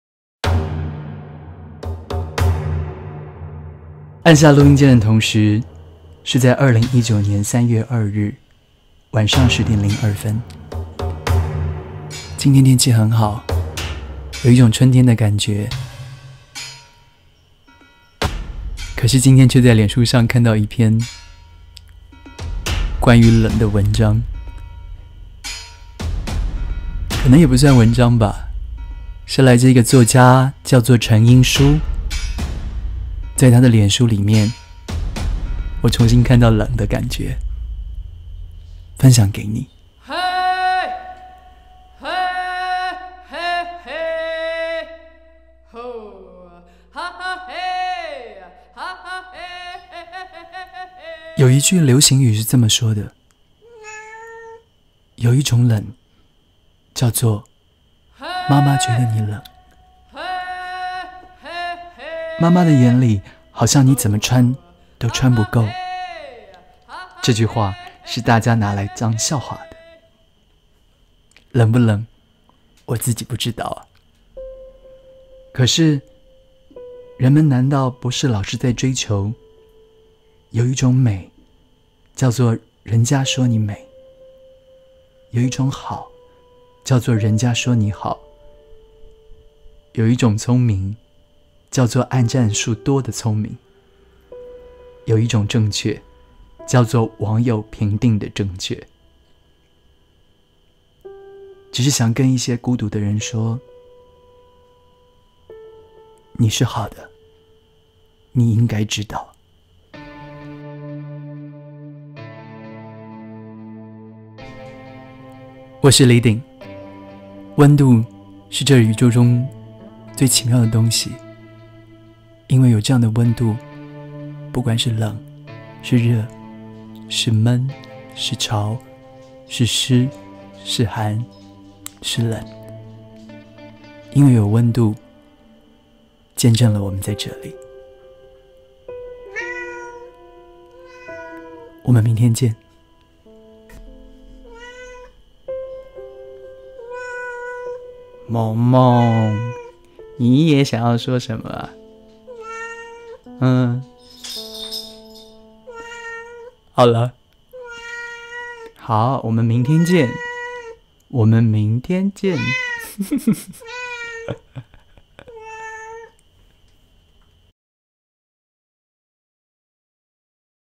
李鼎＿為你朗讀「成英姝的冷」
答應大家不止朗讀書
也朗讀一封信
一篇文章
或是如今天摘錄的一段臉書

這是來自作家
2019年3月2日下午四點
在臉書發布的134個字

分享給所有孤獨的人

第六集「螺絲狗愛上愛情海的魚了」

關於成英姝：
成英姝

　　清華大學化學工程系畢業。曾任環境工程師、電視節目企畫製作、電視電影編劇、電視節目主持人、勁報出版處處長、大成報創意總監兼整合行銷部總經理等職。現專事寫作。

　　在各媒體發表小說、散文、書評、影評；也曾涉獵各種其他形式的藝術創作，出版攝影及繪畫作品，舉辦裝置藝術展。行政院文建會選為二○○○年十大文學人。以《無伴奏安魂曲》獲第三屆時報百萬小說獎首獎。

　　作品包括短篇小說集《公主徹夜未眠》、《好女孩不做》、《私人放映室》、《恐怖偶像劇》、《究極無賴》、《惡魔的習藝》；長篇小說《人類不宜飛行》、《無伴奏安魂曲》、《似笑那樣遠，如吻這樣近》、《地獄門》、《男妲》、《Elegy哀歌》、《人間異色之感官胡亂推理事件簿》；散文集《女流之輩》、《戀愛無用論》；攝影遊記《魔術奇花》。整理父親口述歷史《我曾是流亡學生》以及塔羅心理書《神之手》等。